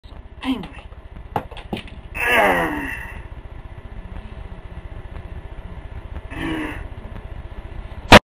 The Tick punches camera
The Tick punches his camera out of anger in order to demonstrate to the trolls how upset he is with them.